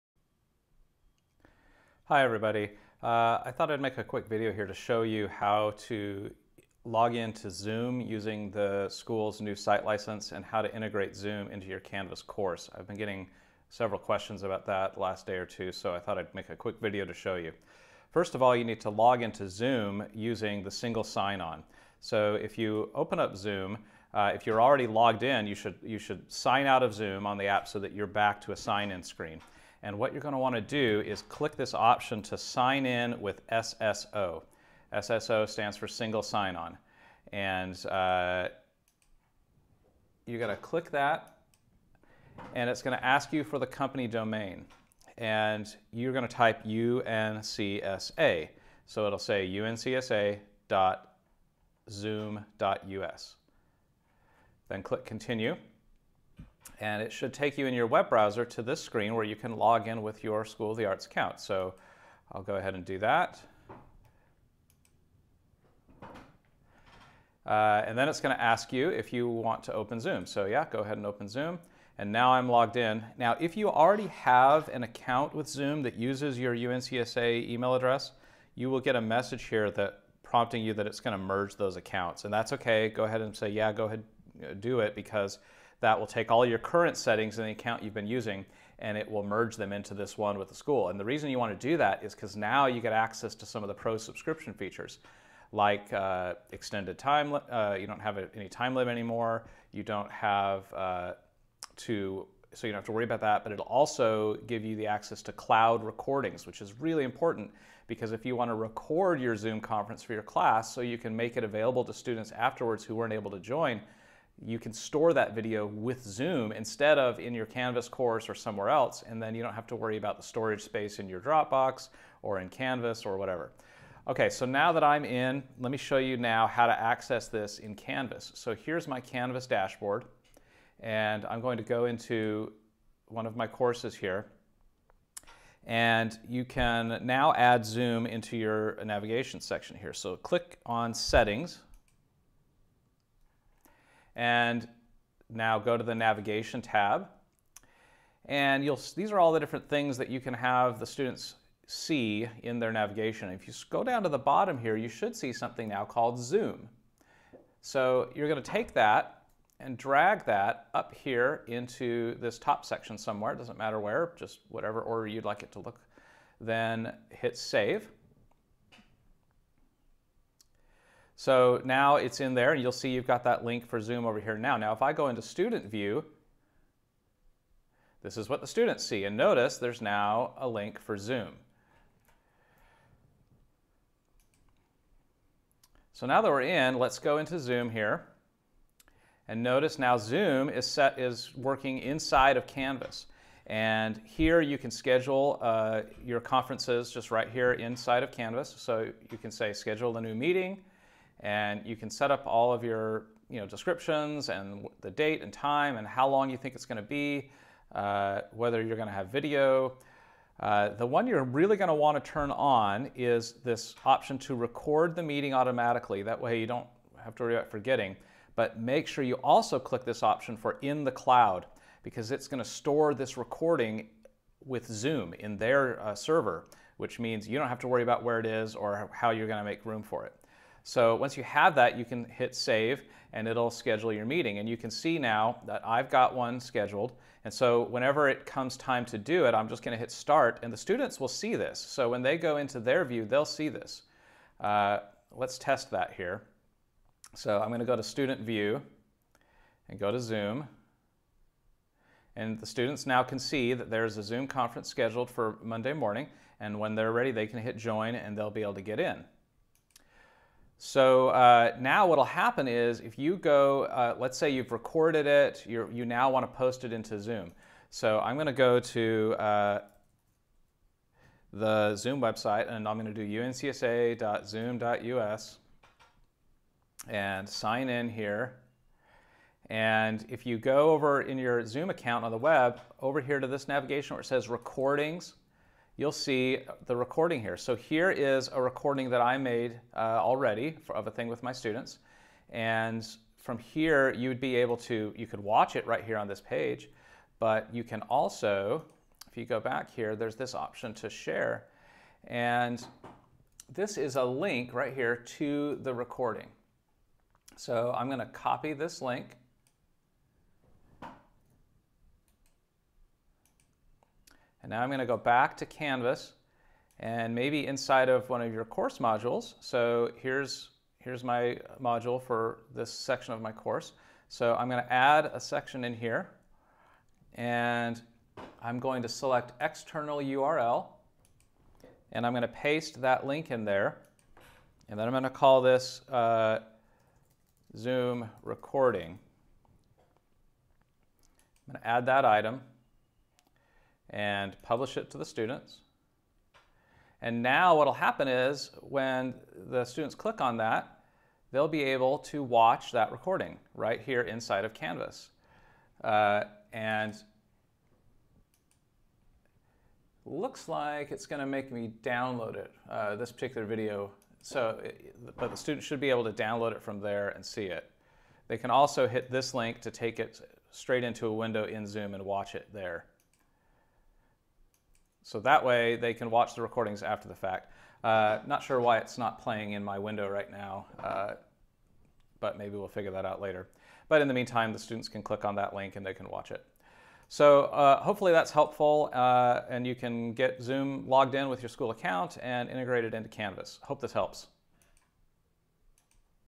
Using Zoom at UNCSA with Canvas
A demonstration on how to sign into Zoom using your UNCSA account and integrate Zoom into your Canvas course.